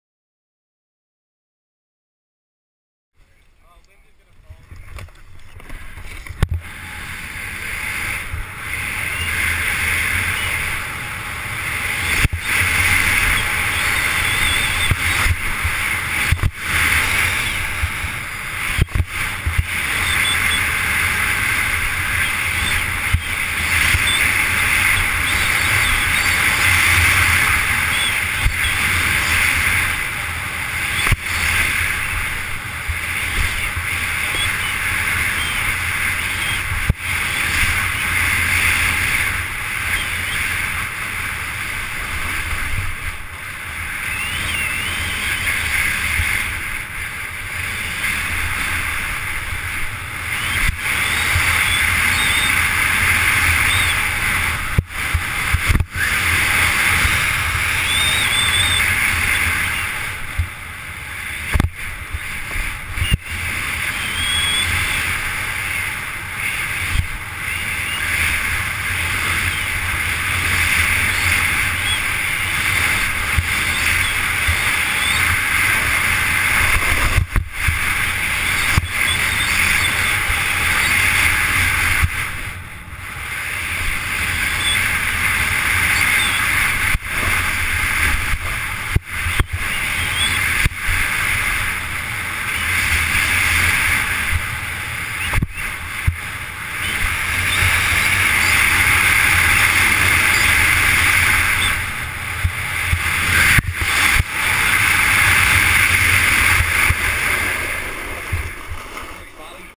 X Games 2011 Emilie Aubry
Aspen, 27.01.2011 filmé pendant les entraînements